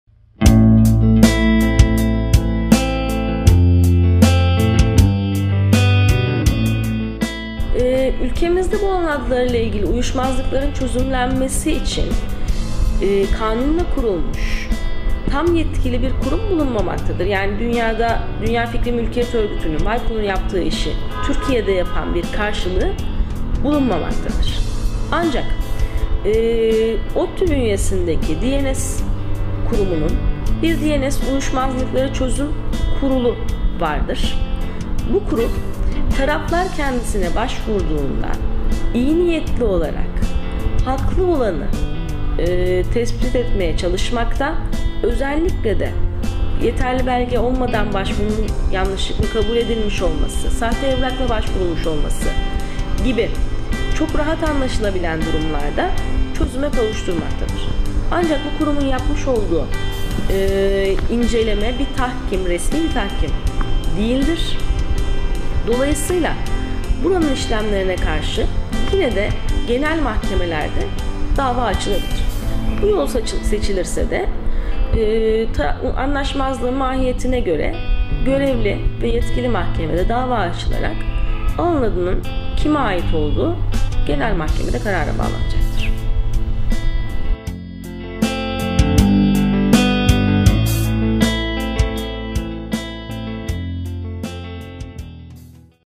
AV. Ceyda Cimilli AKAYDIN [HukukTV.com]
".tr" uzantılı alan adları ile ilgili uyuşmazlıklar nasıl çözümlenir?
Bu konuda öncelikle nic.tr yönetimine e-posta yolu ile başvurularak uyuşmazlığın çözümü talep edilir. Kurumun uyuşmazlığın kendi çözebileceği sınırların dışında olduğunu belirtmesi durumunda veya sunulan çözümün tatmin edici olmaması halinde genel mahkemelere başvurularak dava yolu ile çözüm aranır.
Detaylı Bilgi İçin Linke Tıklayın.